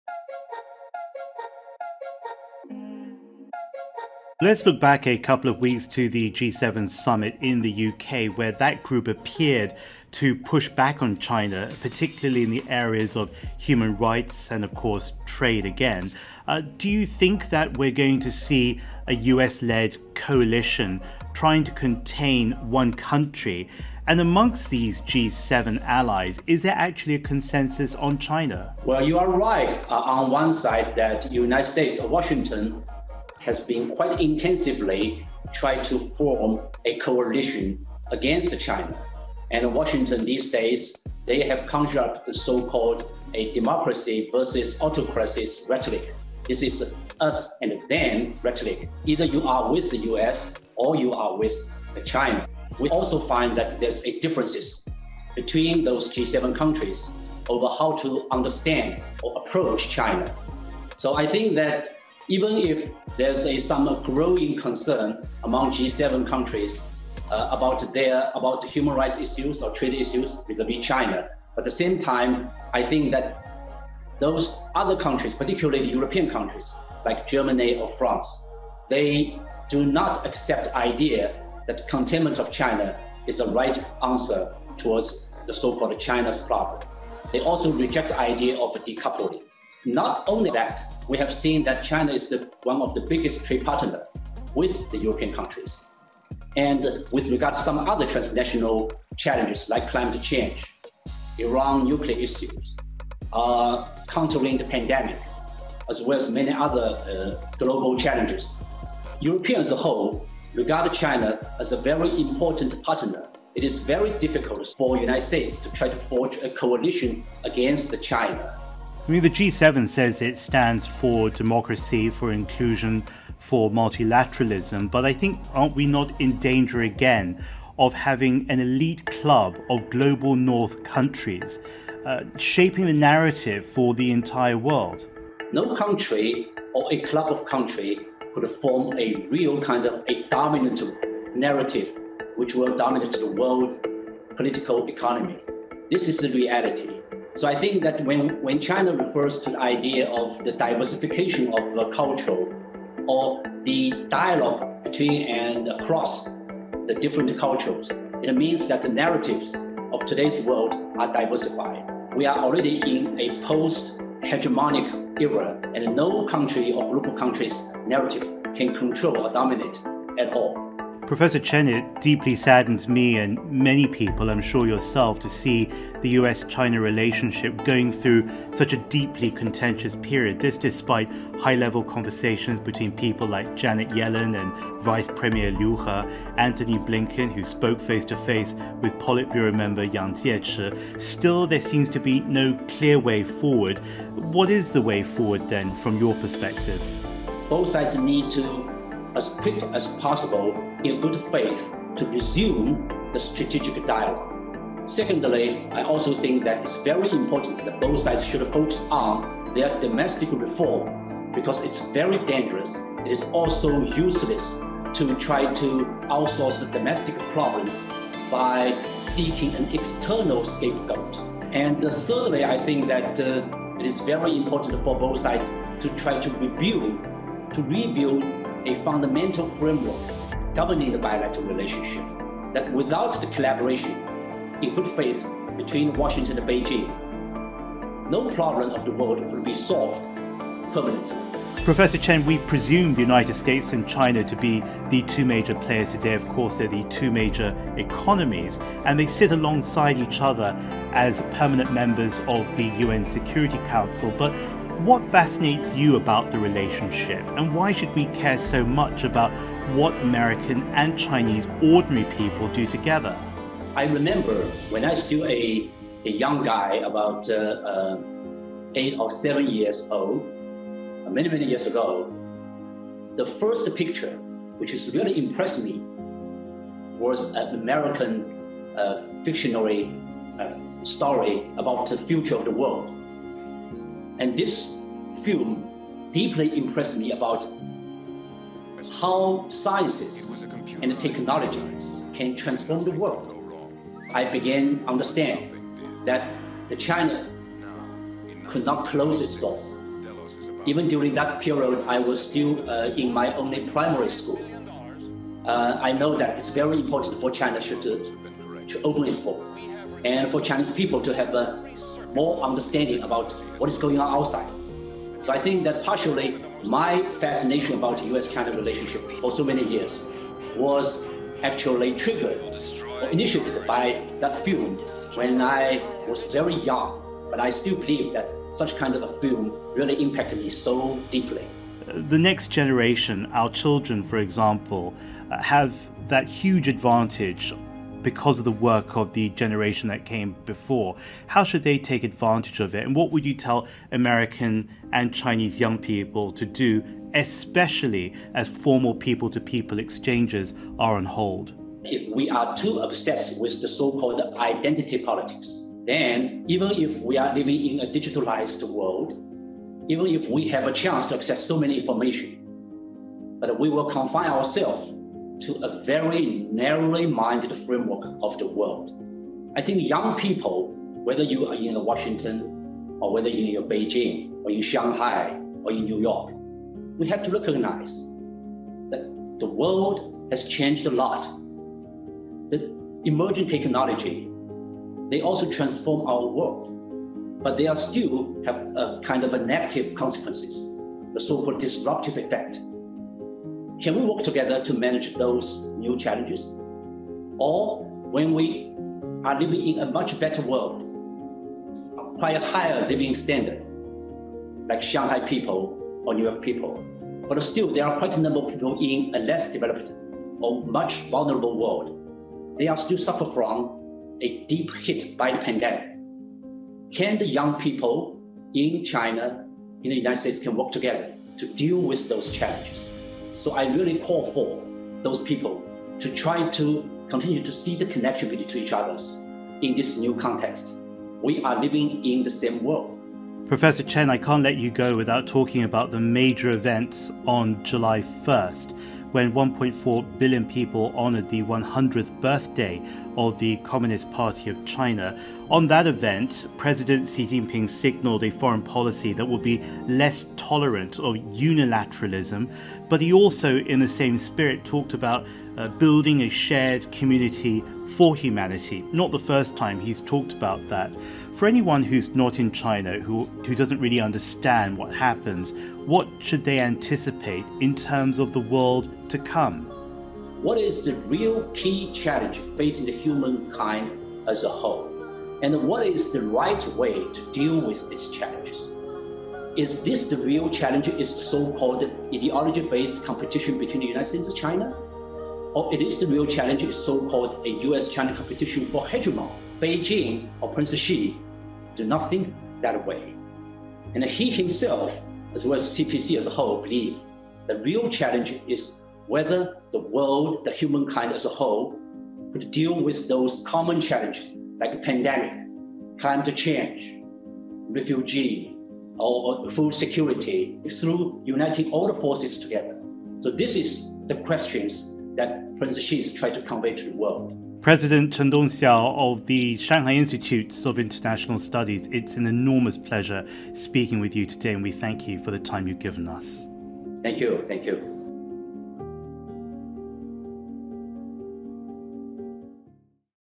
We Live In the Same World | The Pacific Dialogue
Tensions between the world's two leading economies show no signs of easing.

Professor Chen Dongxiao, from the Shanghai Institutes of International Studies, speaks with our Host James Chau about a new vision for this relationship.